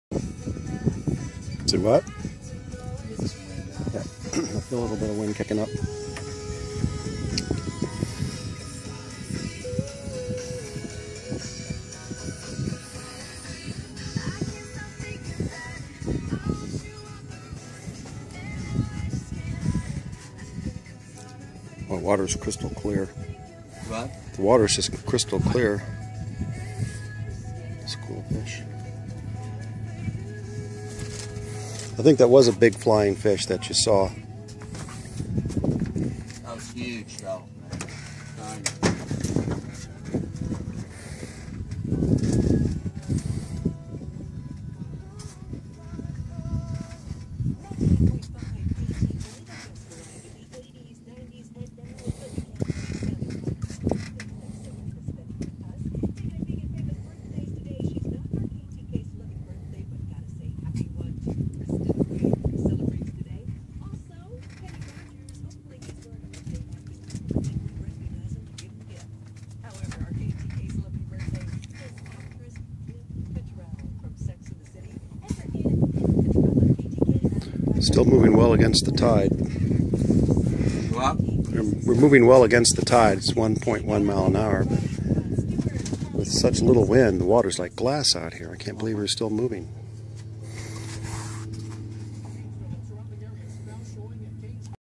Pontoon Sailer
Pontoon Sailer. Experimental sail rigging of 24" pontoon boat. In calm sea, with barely a wind the sail moved the boat against the tide. These are the golden clear shallow waters of Homosassa Bay, Florida west coast (Gulf of Mexico). We are sailing past Saint Martins Keys. Notice the corals and sponges in these pristine waters.